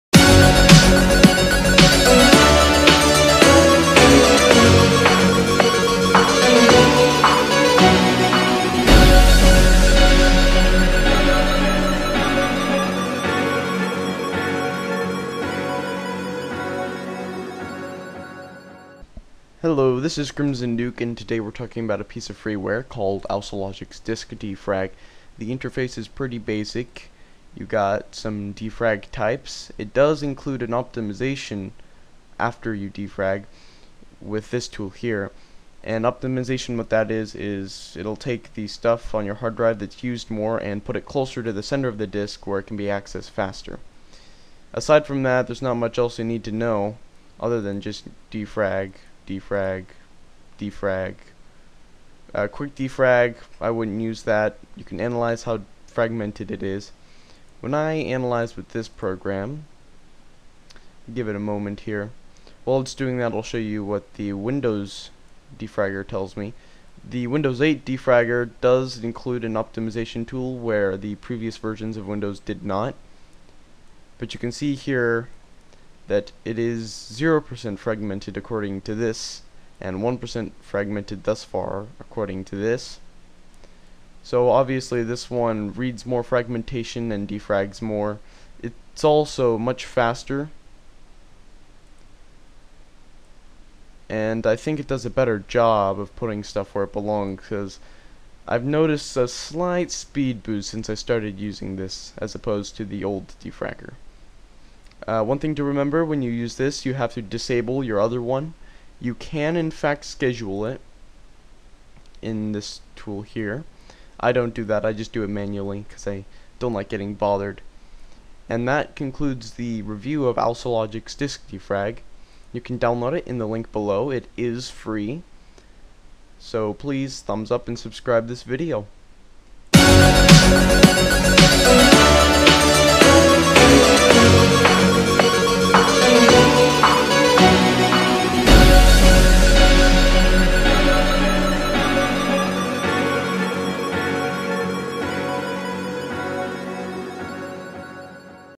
Best FREE disk defragger
Auslogics Disk Defrag is better than all the windows defragers, faster, and includes an optimization tool where only Windows 8 has something similar. (Do not use for solid state drives.)